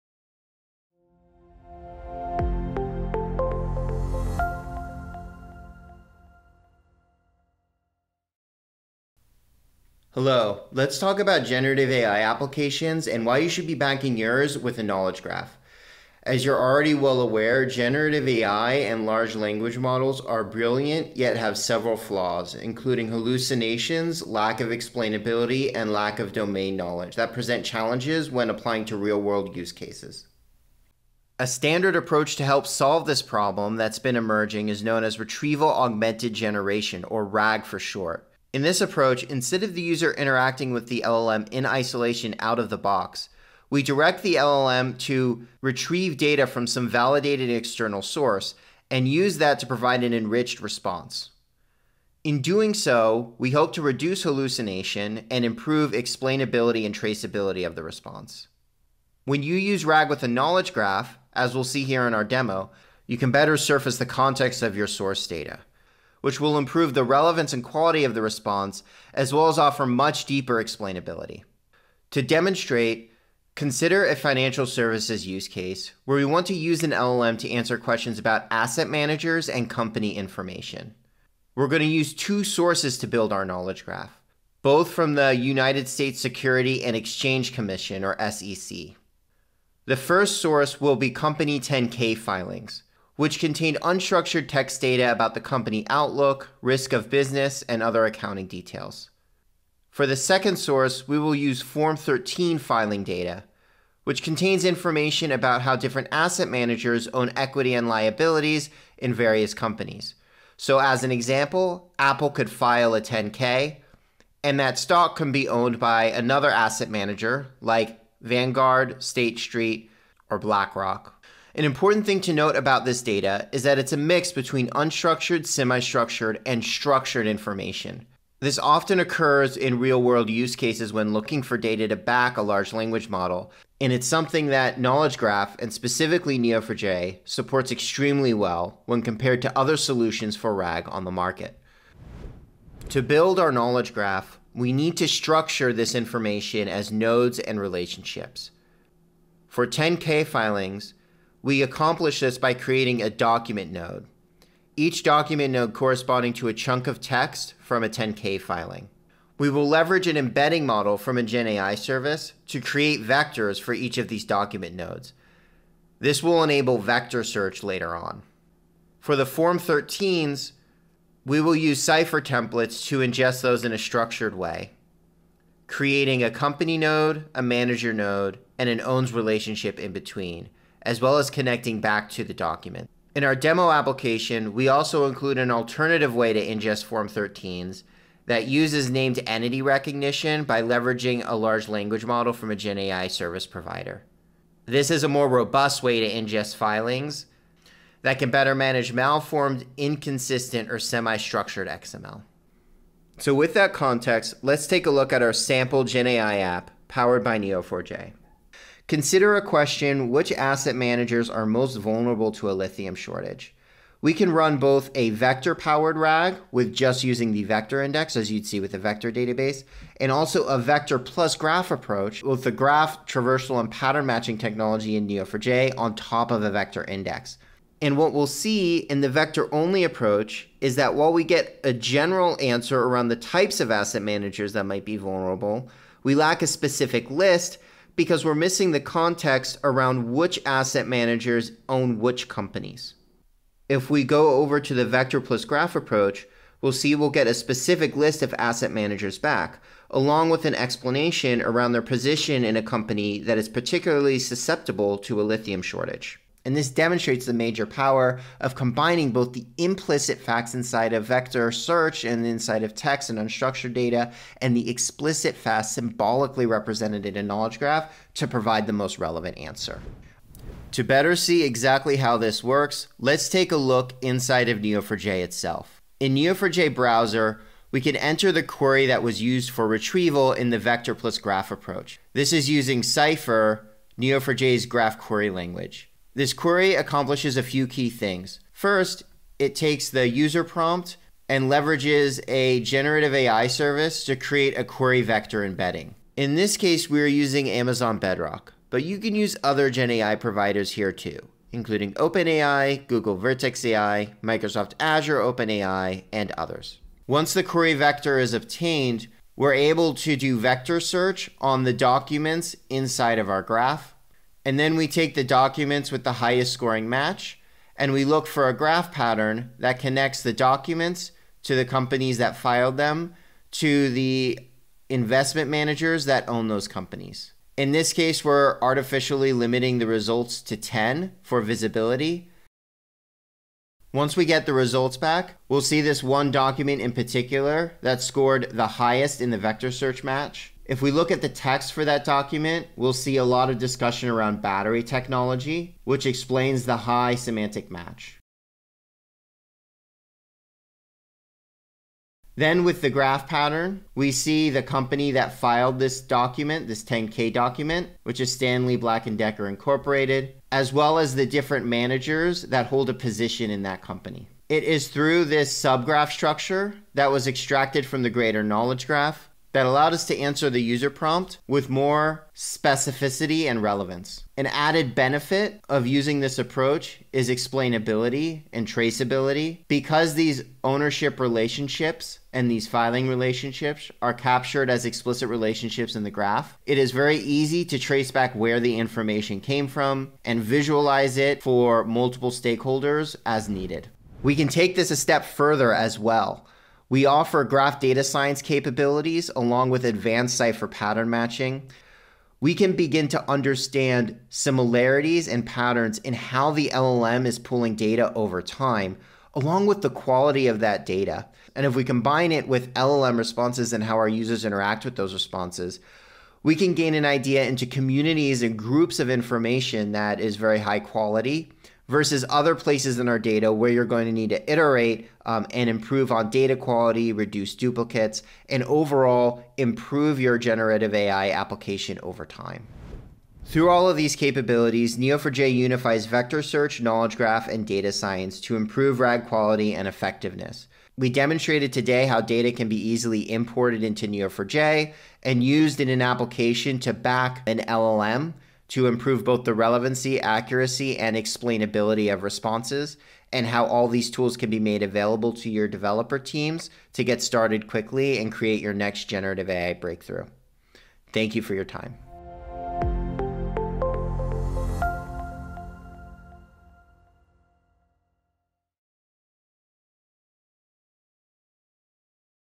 Build Your Next GenAI Breakthrough with Neo4j
See how Neo4j's industry-leading knowledge graph powers cutting-edge Generative AI (GenAI) applications.

In this demo, we'll explore a real-world use case to demonstrate how Neo4j’s knowledge graph, combined with vector search, enables superior Retrieval Augmented Generation (RAG). Learn how Neo4j helps overcome common GenAI challenges like hallucinations and lack of explainability while improving the relevance and quality of Large Language model (LLM) responses.

With Neo4j, you can:

-Build a grounding knowledge graph from structured and unstructured data
-Use graph pattern matching with vector search to provide deeper domain context, enhancing the relevance and explainability of LLM responses
-Apply advanced graph analytics, such as Graph Data Science, to gain -valuable insights into grounding data, allowing you to continually analyze, monitor, and improve grounding data quality over time.